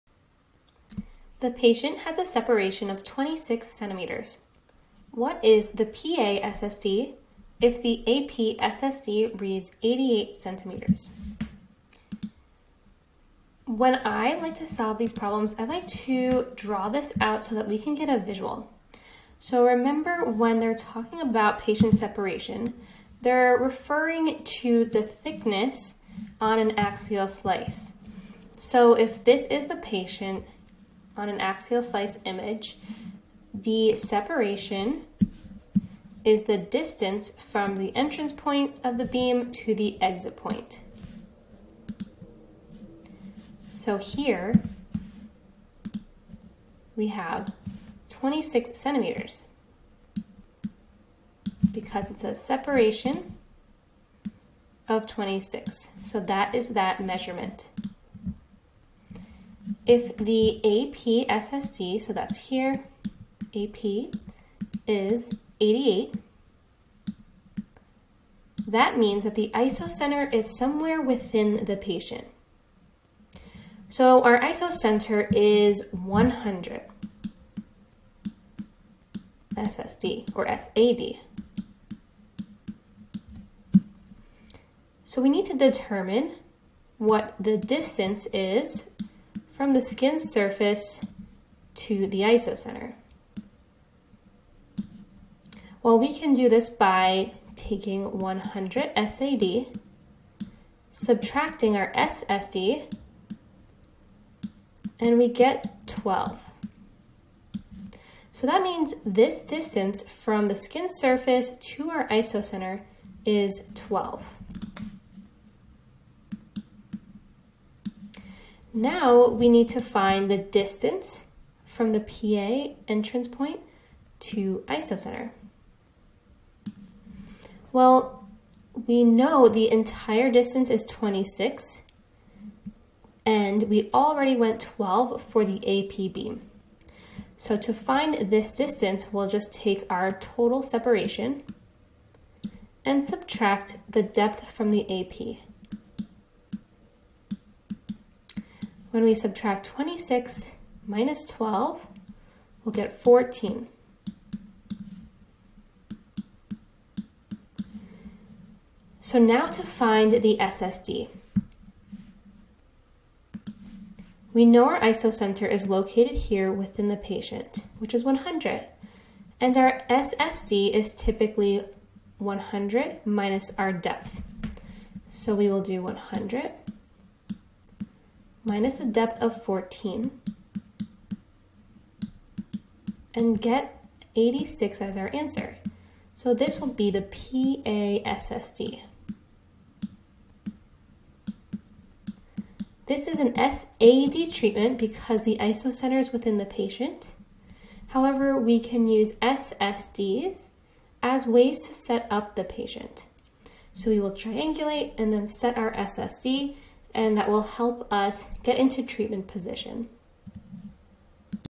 Determining SSD
The source to skin distance (SSD) can be used to assist with patient setup in radiation therapy. Some questions may ask us to find an SSD based on information. This question gives us the patient separation and the AP SSD. Now, we need to determine the PA SSD. This takes a few steps to solve! For more help with prep for the radiation therapy registry, check out my course If you would like 1:1 tutoring apply